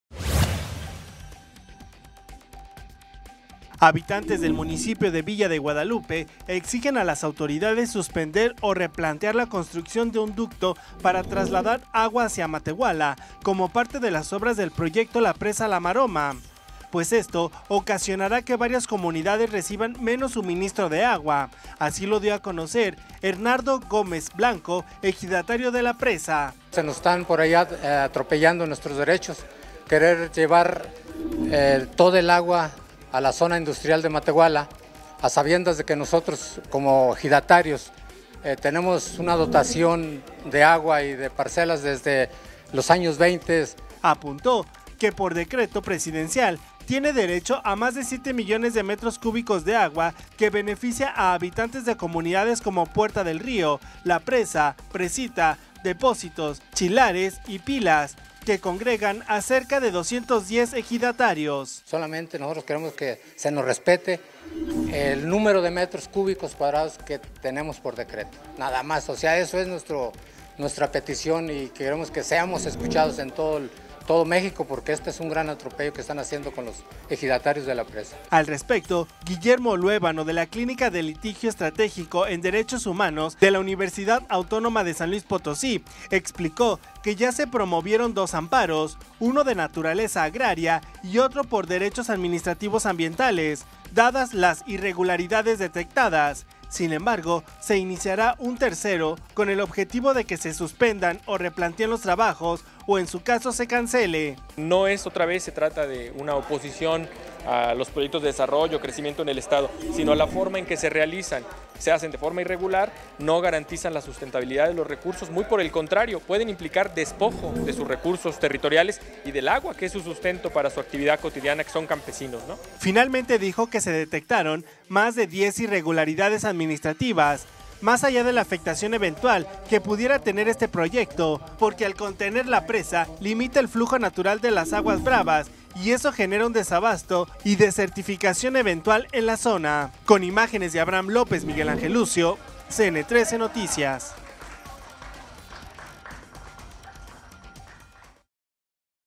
Exigen suspender ducto "La Maroma"
Habitantes del municipio de Villa de Guadalupe